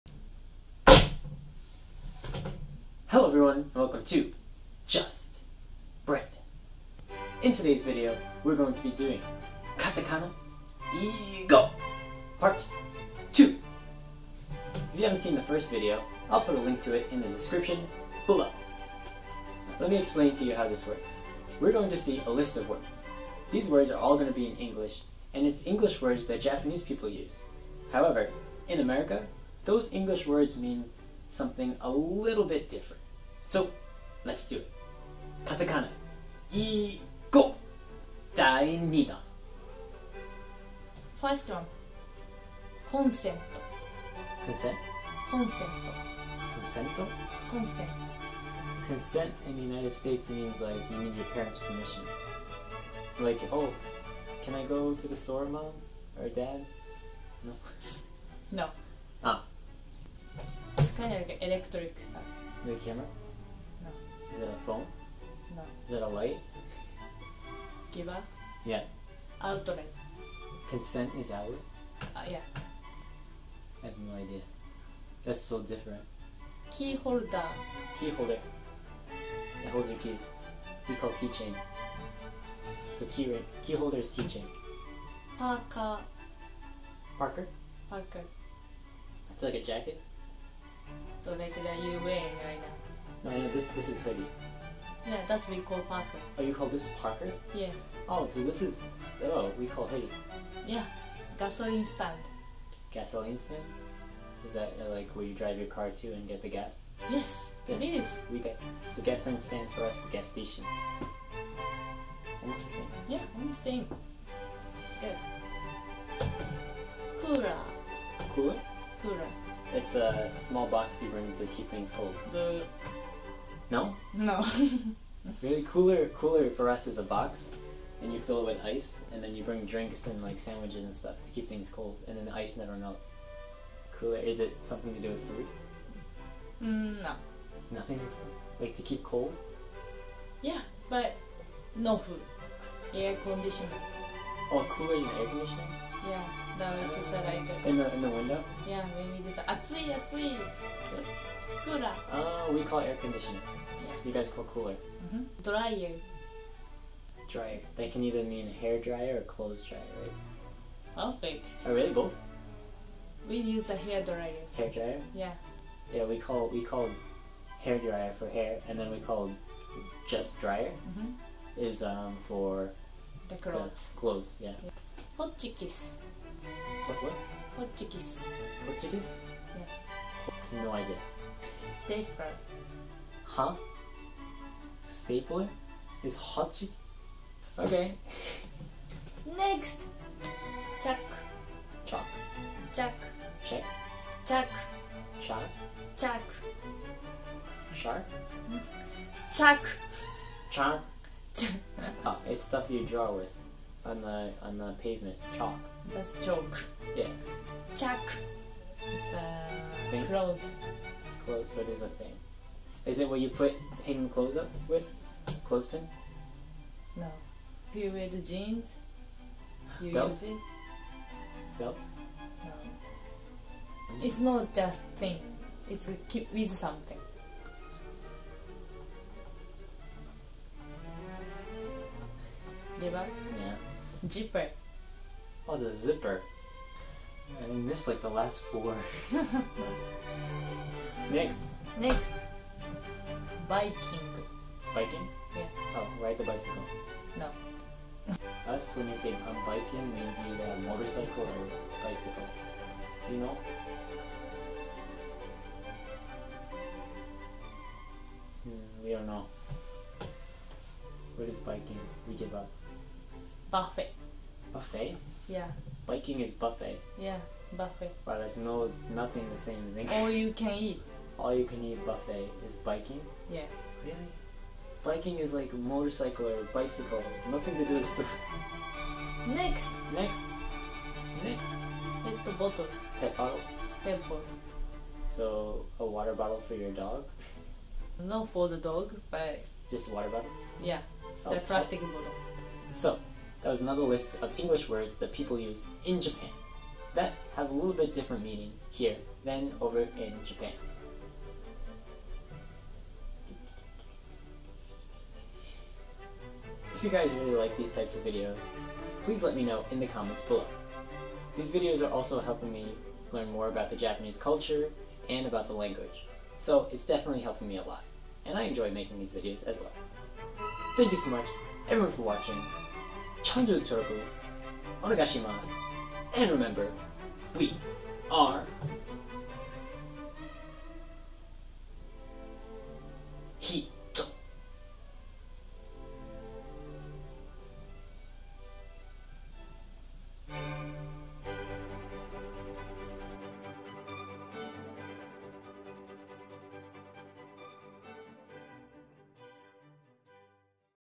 カタカナ英語 第２弾 / Katakana Let's Go Part 2
Brandon is back with another Japanese Lesson!  Today he will revisit Katakana Let's Go!

カタカナ英語！！ / Katakana Let's Go!!